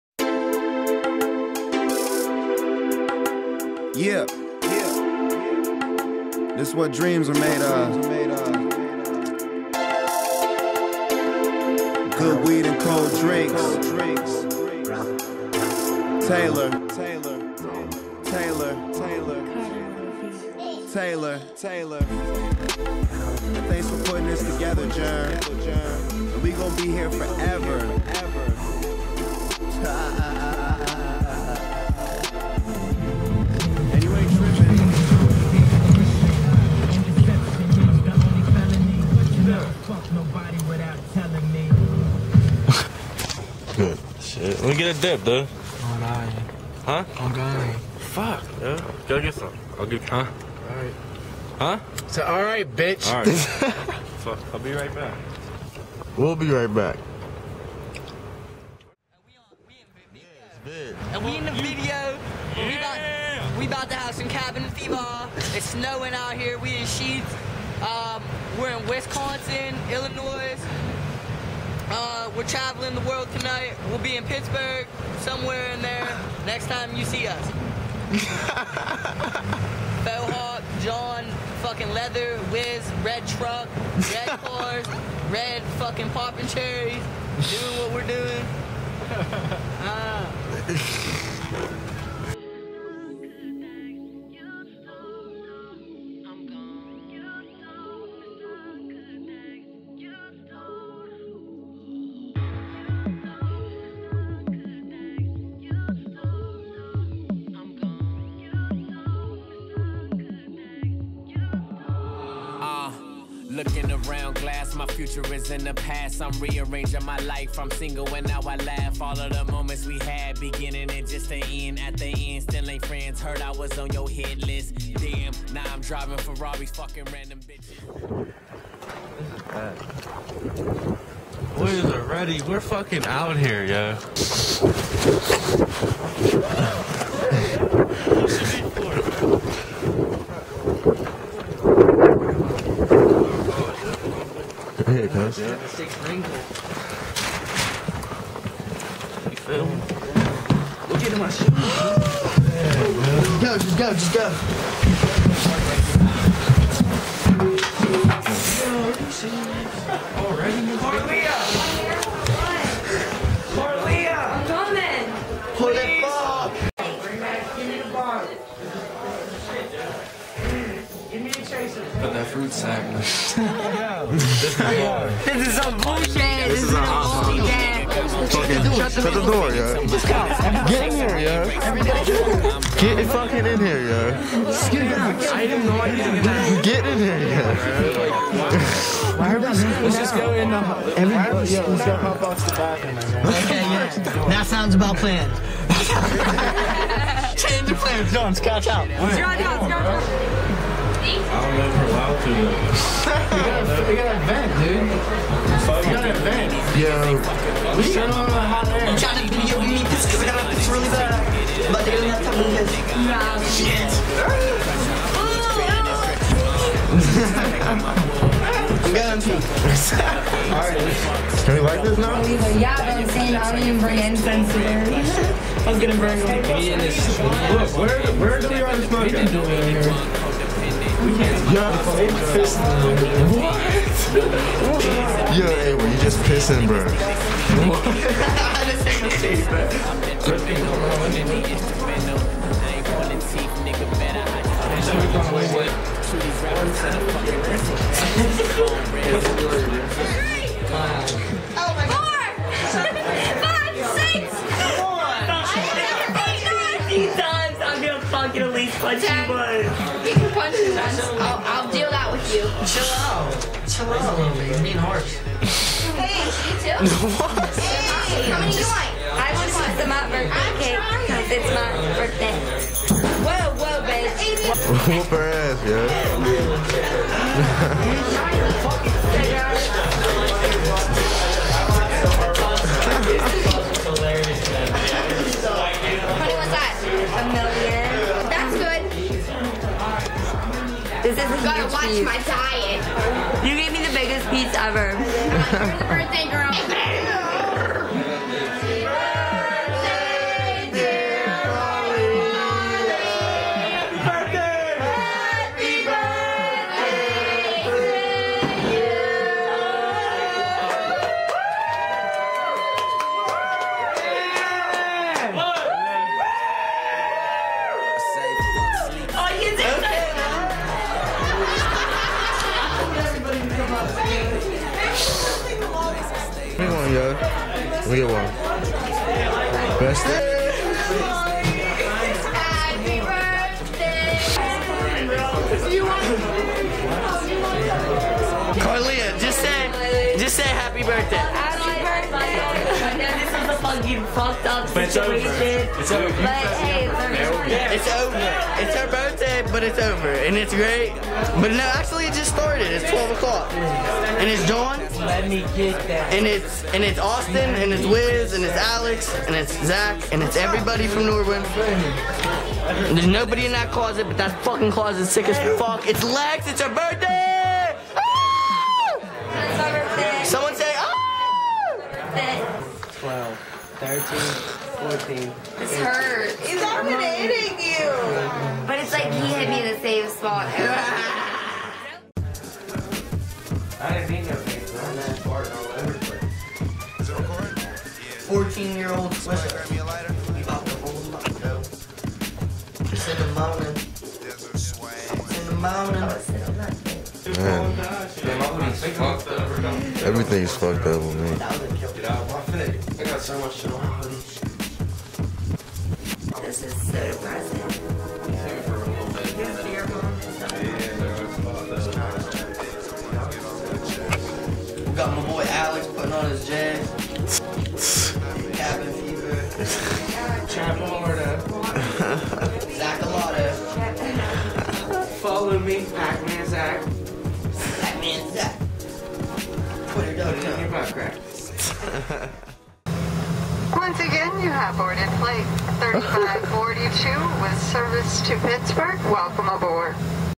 OverHill Ep.5 - Cabin Fever
Me and my Bro's partying up 7 Springs for friends BirthDay